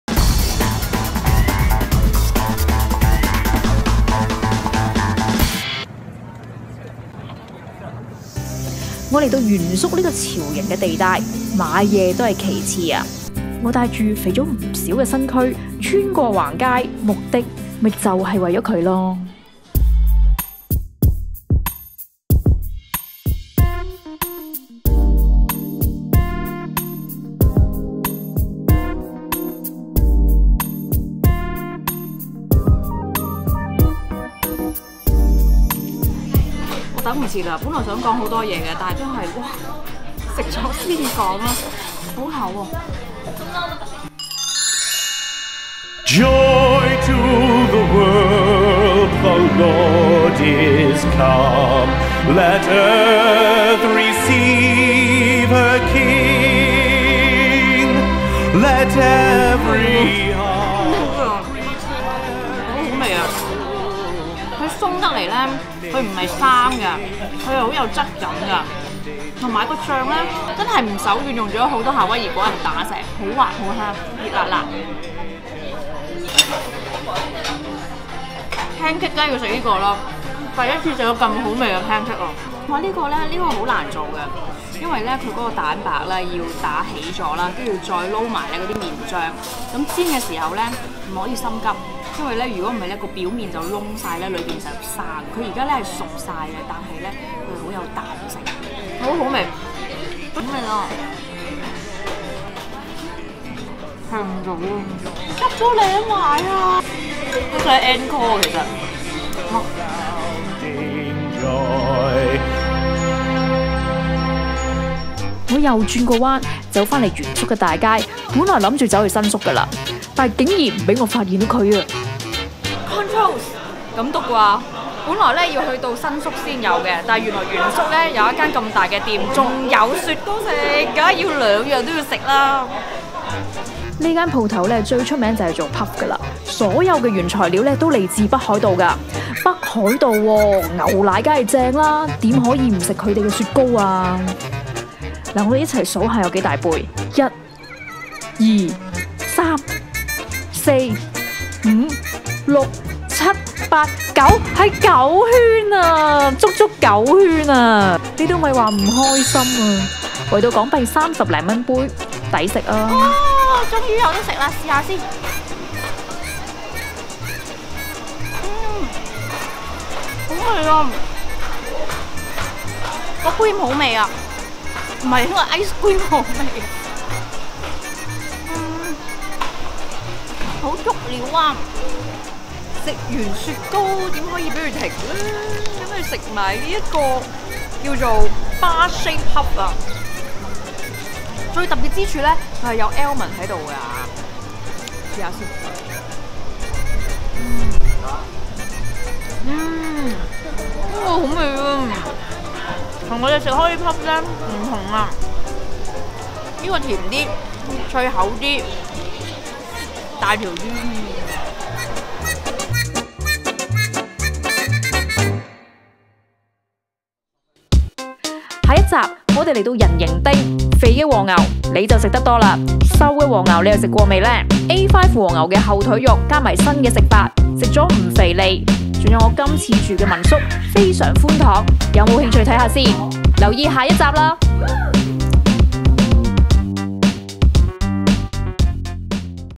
《Kit巡東京》第二集- 原宿神級Pancakes、足9圈“北海道牛乳雪糕、吃上癮便利店布甸
《Kit巡東京》第二集，我去了原宿尋訪真正的神級 Pancakes, 仲有點可以唔食既"足9圈“北海道牛乳雪糕和杏仁脆Puff. 肥肉是這樣鍊成的，但點可以錯過呀！kit巡東京